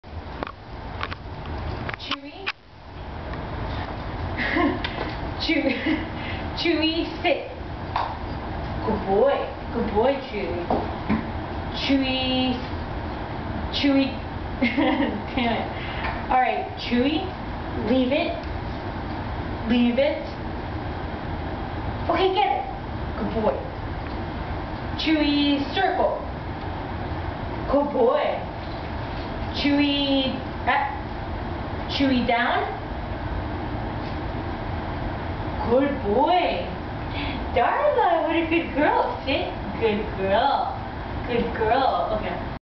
Chewie tricks so far
Miniature Australian Shepherd puppy doing cute tricks